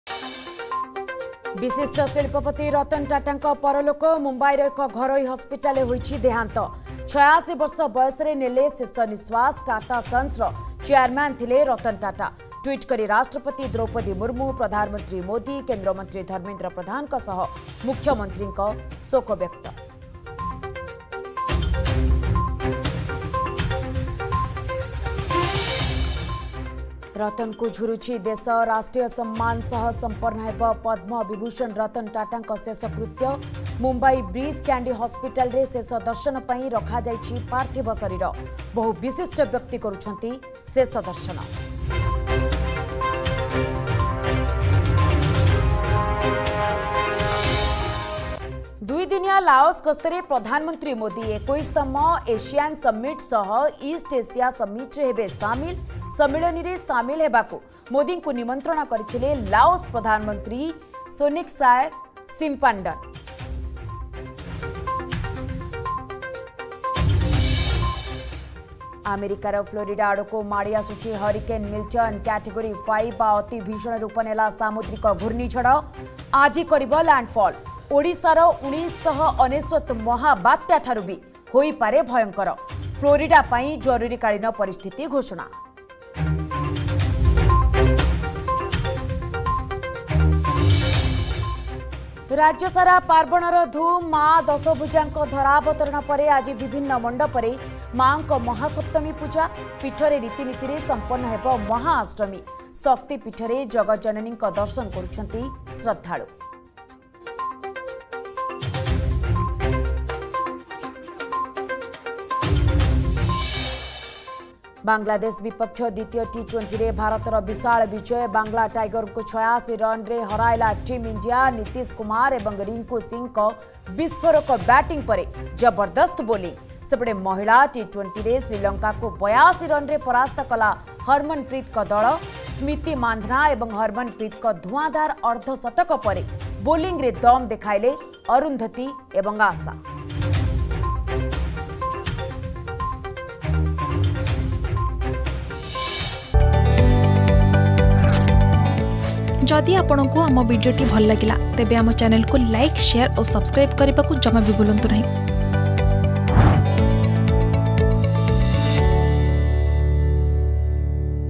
Headlines 9am || 10 Oct 2024 || Argus News
+ବିଶିଷ୍ଟ ଶିଳ୍ପପତି ରତନ ଟାଟାଙ୍କ ପରଲୋକ
+ରତନଙ୍କୁ ଝୁରୁଛି ଦେଶ
+ଦୁଇ ଦିନିଆ ଲାଓସ ଗସ୍ତରେ ପ୍ରଧାନମନ୍ତ୍ରୀ ମୋଦି
+ଆମେରିକାର ଫ୍ଲୋରିଡ଼ା ଆଡ଼କୁ ମାଡ଼ି ଆସୁଛି ହରିକେନ୍ ମିଲଟନ୍
+ରାଜ୍ୟସାରା ପାର୍ବଣର ଧୁମ୍
+ବାଂଲାଦେଶ ବିପକ୍ଷ ଦ୍ବିତୀୟ ଟି-୨୦ରେ ଭାରତର ବିଶାଳ ବିଜୟ